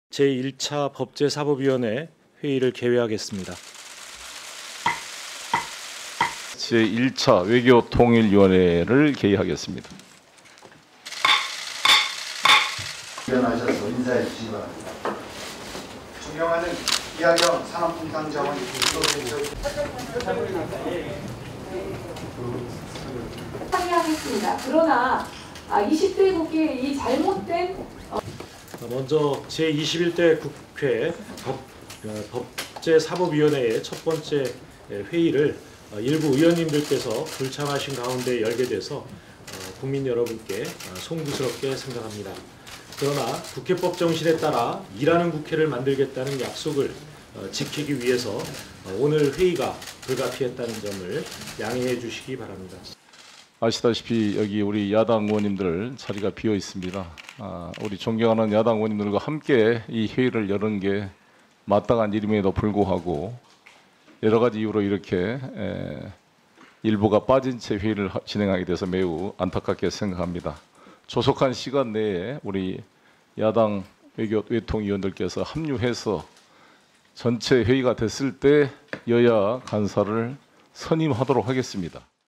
21대 첫 상임위부터 ‘반쪽짜리’…與,더 커진 독주 부담
16일 오후 국회에서 윤호중 법세사법위원장과 더불어민주당 등 범여권 정당 소속 의원들이 참석하고 미래통합당 의원들이 불참한 가운데 법사위원회 첫 전체회의가 진행되고 있다.